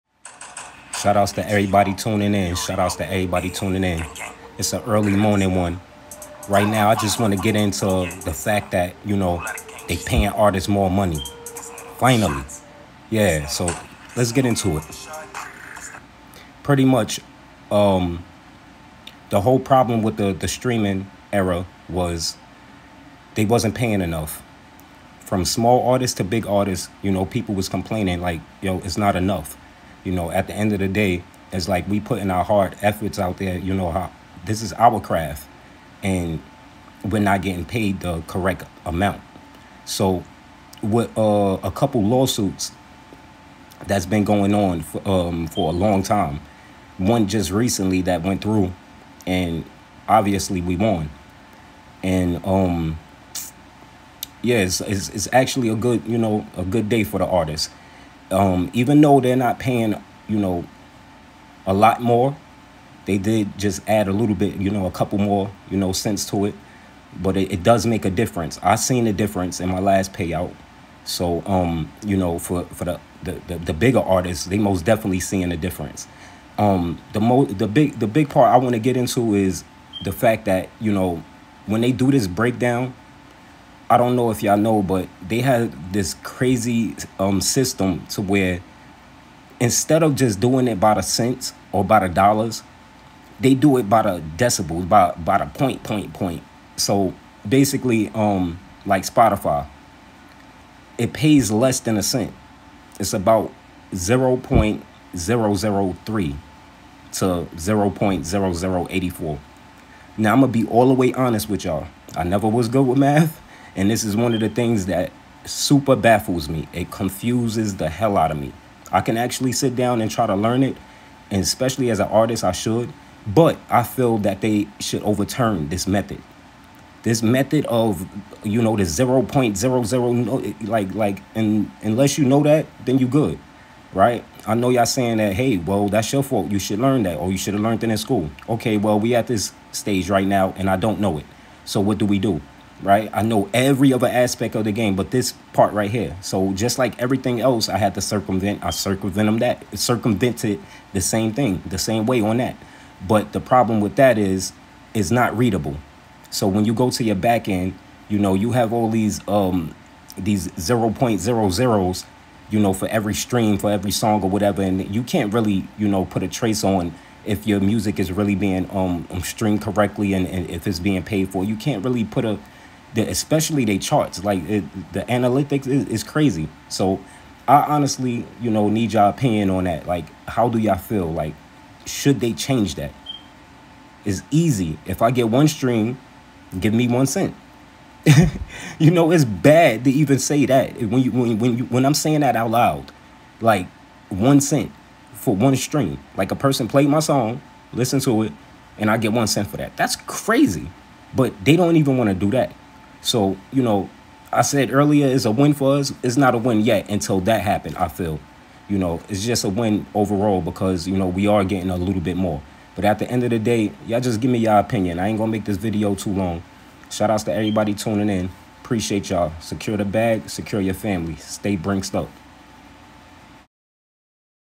Streaming services paying more
A small change for music artist when it comes to streaming payouts